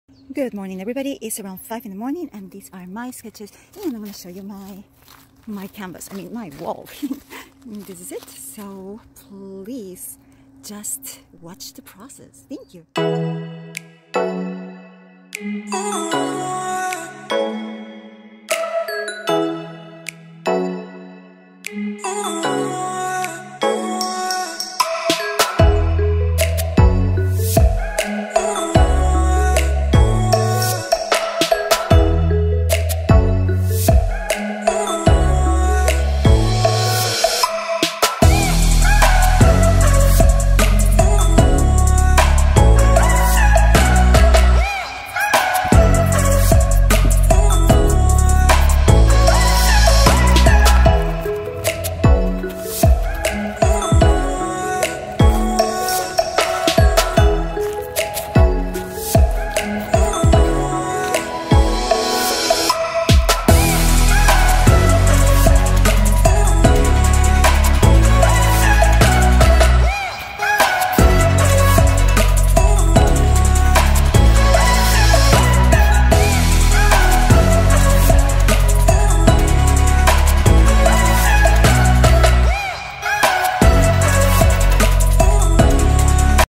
Mural art Custom made in back yard block wall in a Scottsdale home Arizona Art
Cacti mural canvas art for a home in Scottsdale spray paint on wall backyard concrete-block fences stucco wall fences in arizona stucco, brick, stone back fence wall idea patio decor murals.